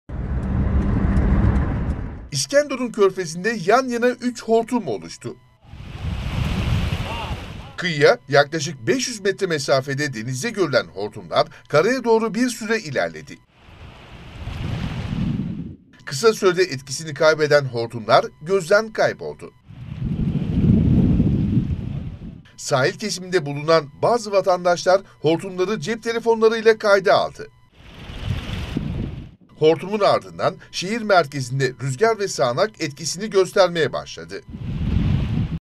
İskenderun Körfezi'nde aynı anda 3 hortum
Yurdun bir çok noktasında etkisini gösteren kötü hava, İskenderun'u da etkisi altına aldı. İskenderun Körfezi'nde yan yana 3 hortum oluştu. Hortumun ardından şehir merkezinde rüzgar ve sağanak başladı.